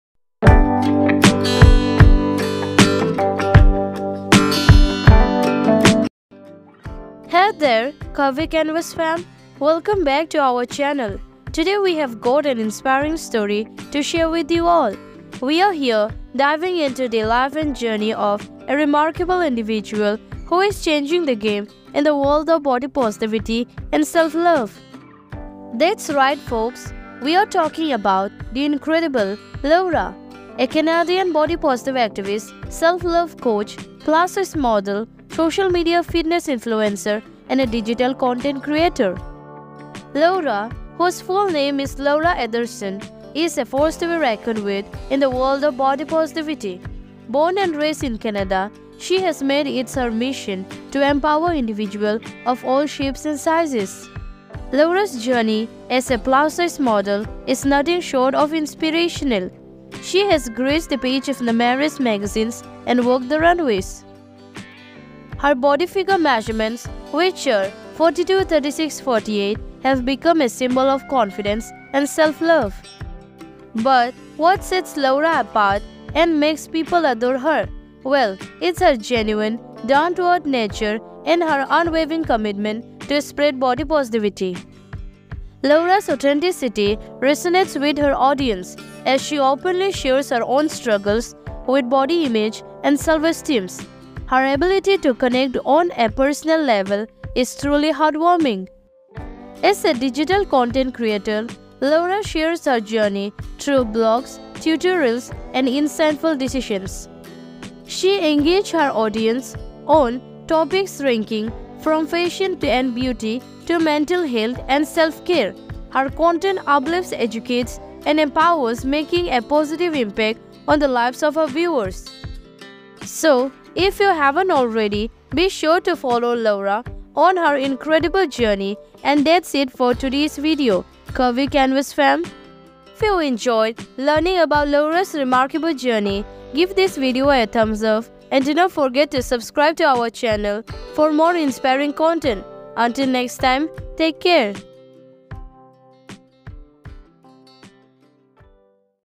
Laura gymthickmami | Canadian Super Curve Model | Big Size Insta Model Wiki | Content Creator
Join us as we dive into the inspiring journey of Laura Anderson, a Canadian force of positivity in the world of body acceptance. Laura, a plus-size model, social media fitness influencer, and digital content creator, is changing the game by promoting self-love and empowerment for all. Discover her story, her impact, and how she's making a difference in the lives of countless individuals.

In-frame Model: Laura
Follow her on Instagram: @gymthickmami